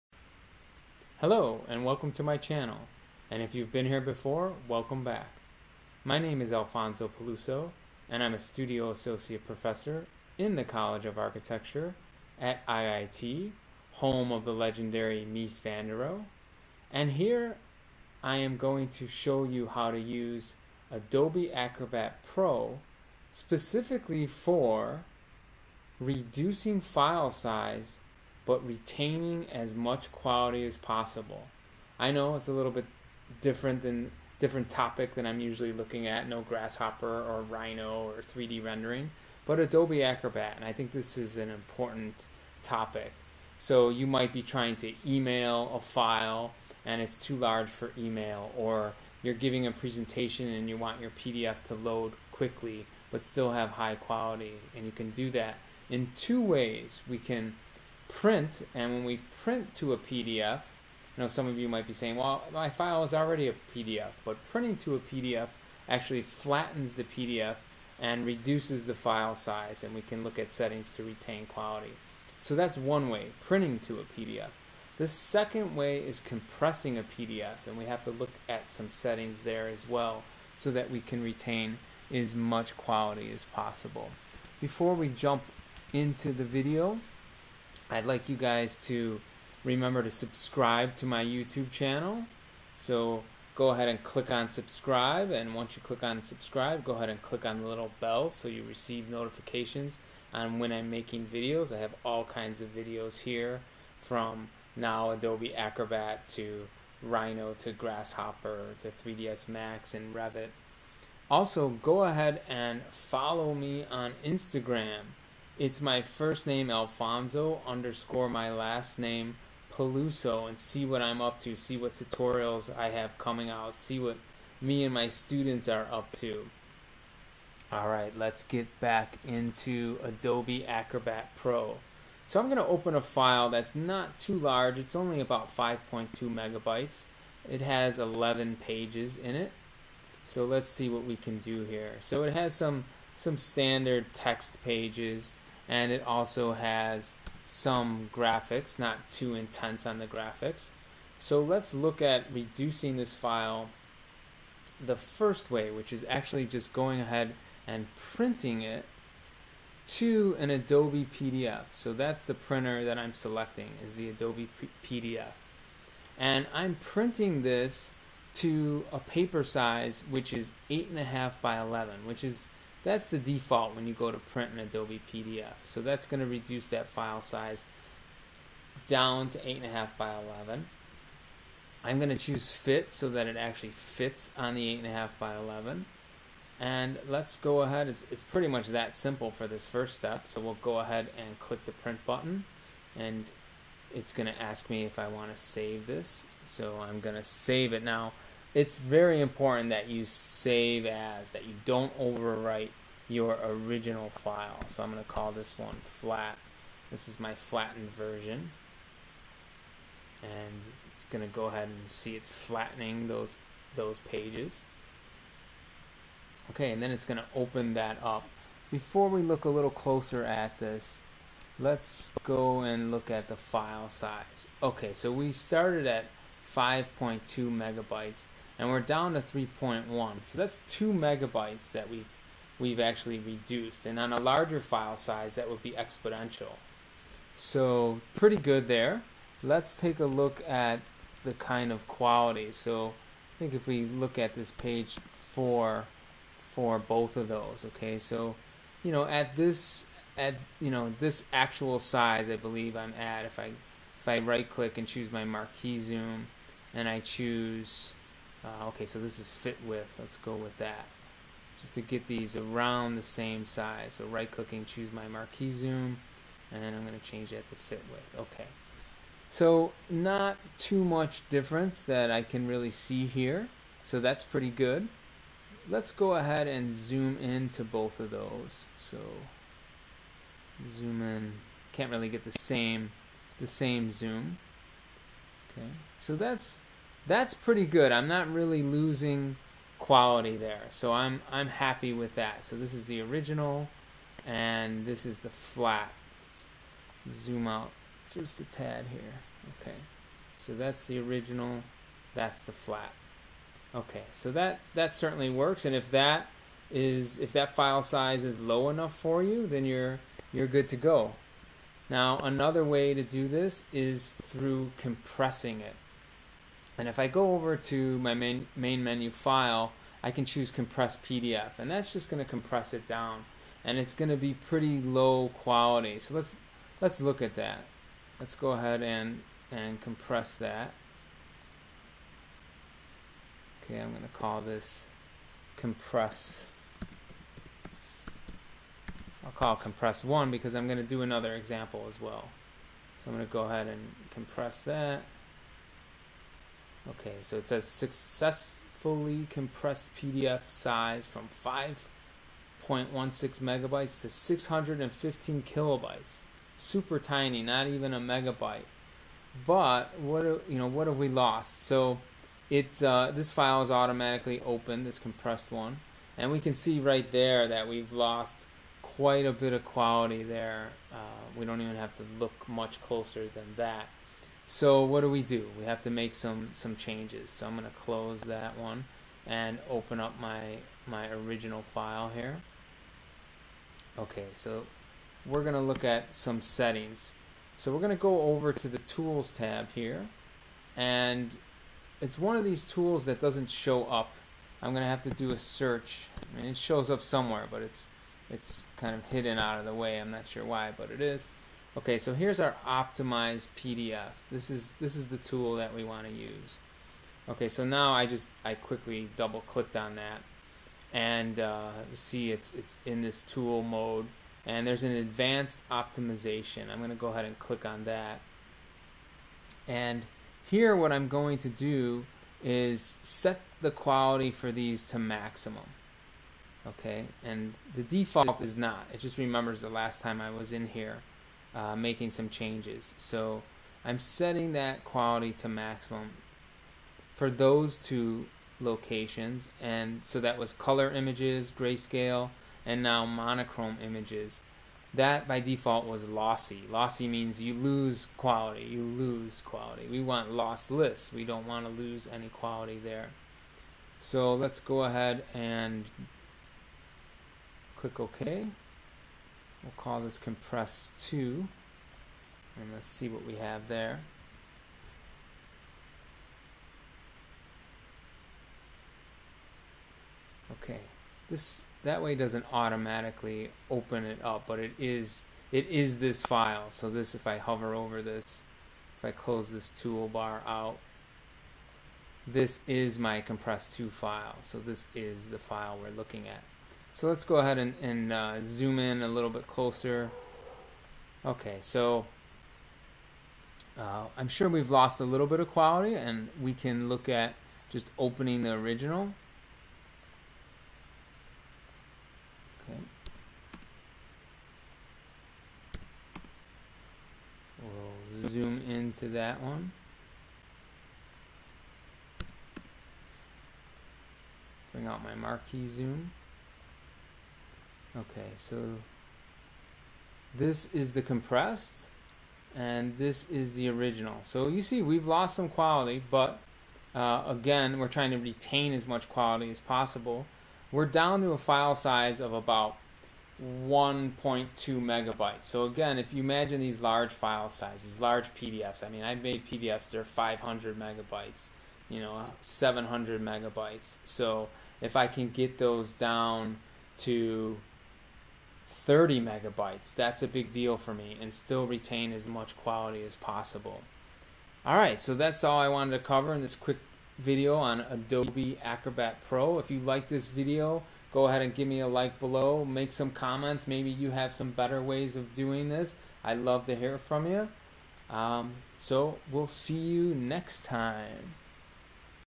Adobe Acrobat File Size Reduction
This video looks at two different methods to reduce file size but retain as much quality as possible.